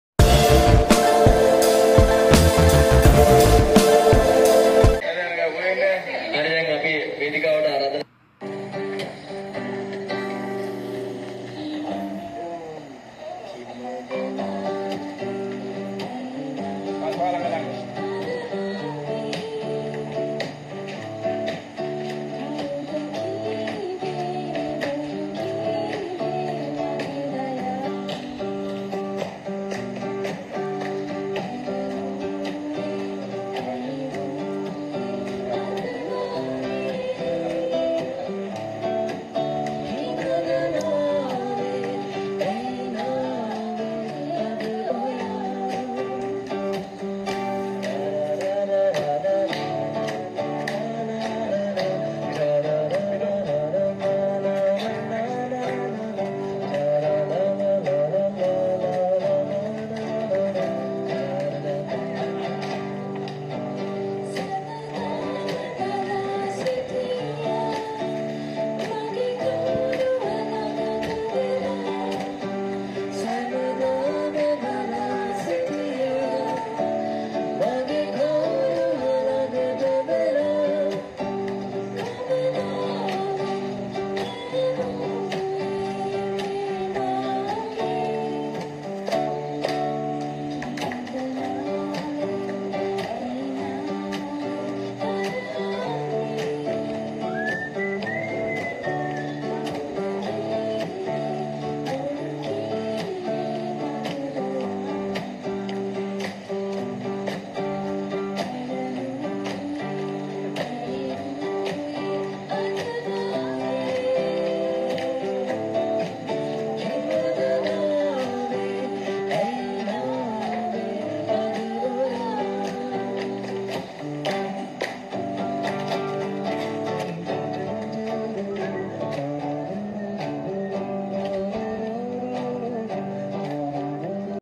ජැක්සන් පවුලේ බාල ලේලි ප්‍රථම වතාවට ලැජ්ජාවෙන් ගයපු අපූරු ගීතය [ Video]
ජැක්සන් පවුලේ බාල ලේලි ප්‍රථම වතාවට ලැජ්ජාවෙන් ගයපු අපූරු ගීතය
Buvini Chapa
Sajitha Anuththara Anthony

Edit             Chabee
Producer   Chabee